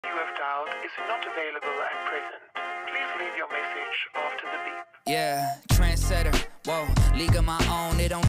Cat? 🥱 Cat with sunglasses 🤩!
Trendsetter cat rocking sunglasses on the beach.